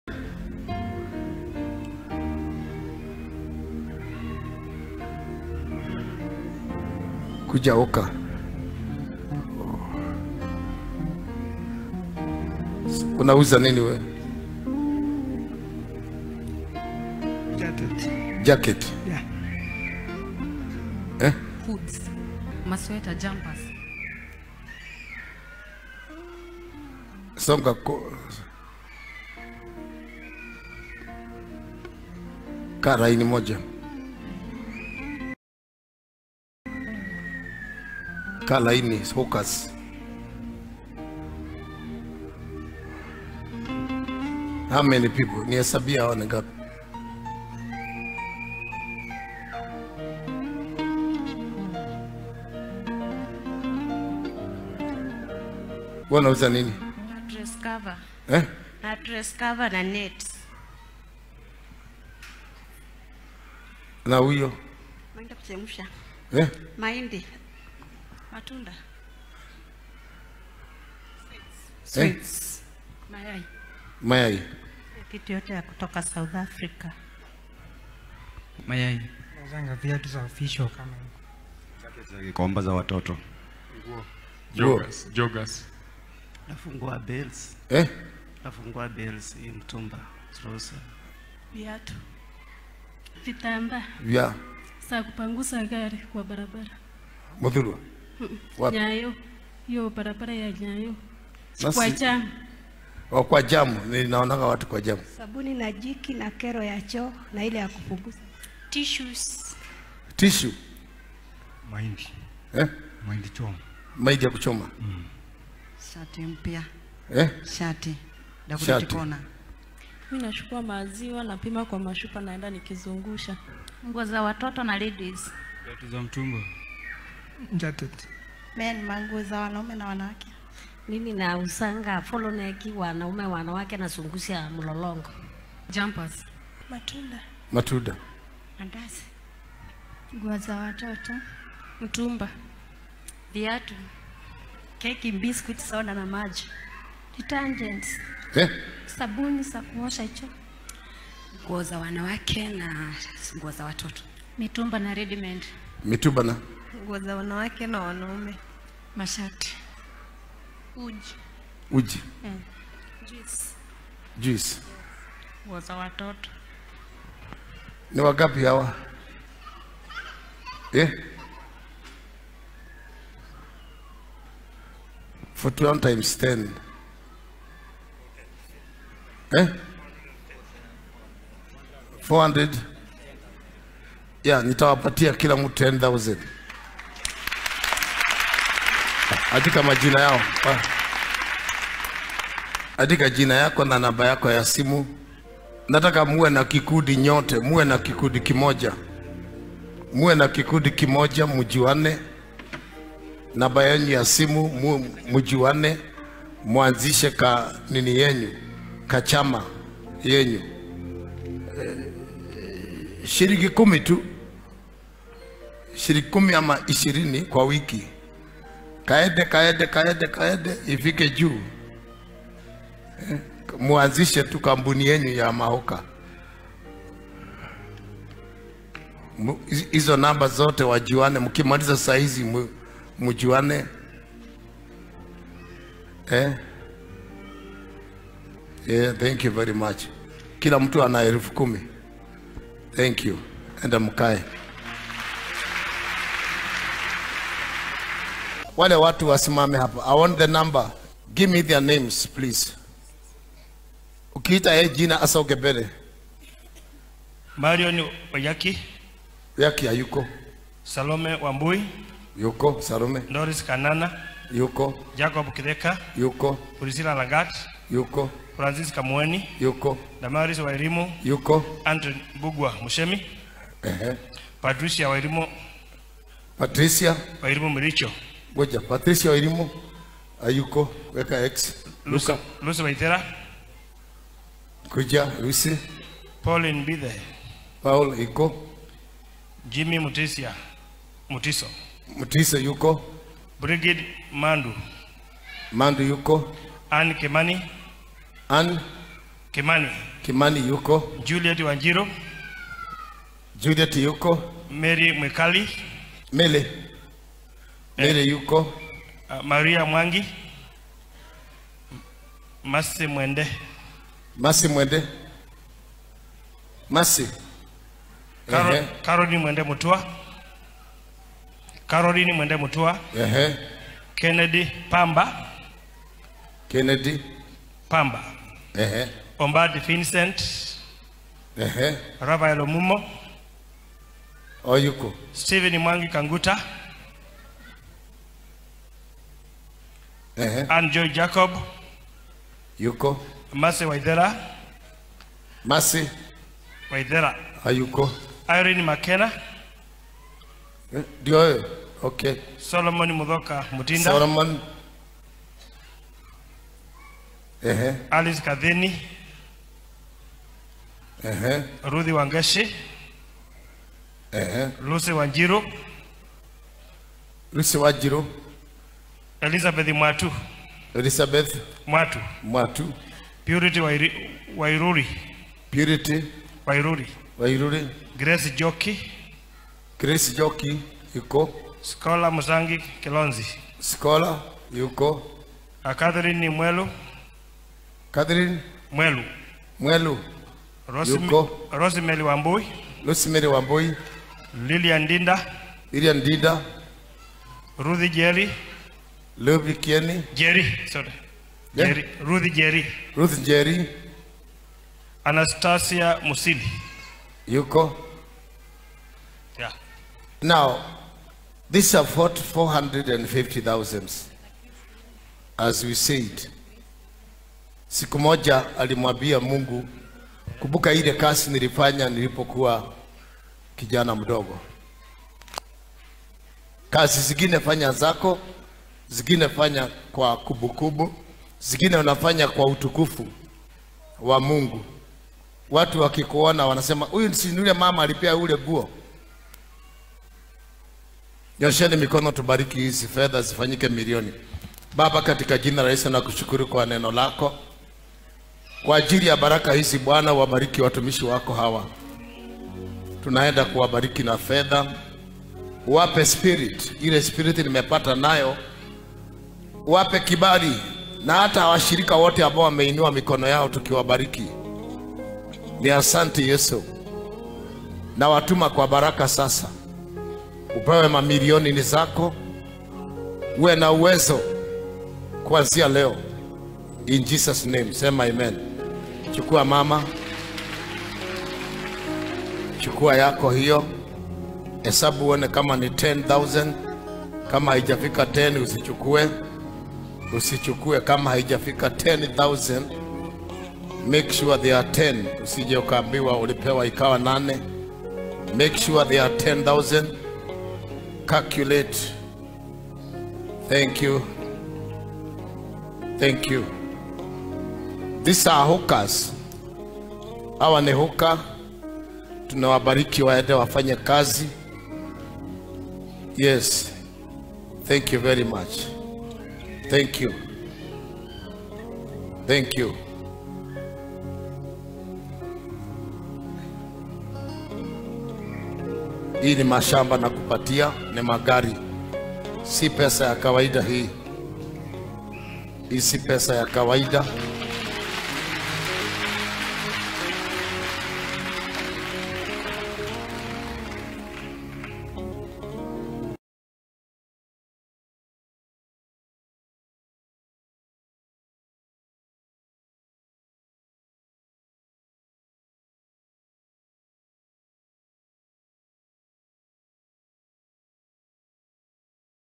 PASTOR NGANGA TRENDING: YOU WON’T BELIEVE WHAT PASTOR NGANGA DID TO HAWKERS IN HIS CHURCH
PASTOR NGANGA TRENDING: SEE COMPASSION GOSPEL IN NENO EVANGELISM CHURCH
Pastor Nganga  Today Gives 450k to Hawkers.
 Rev. Ben Message to Pastor Nganga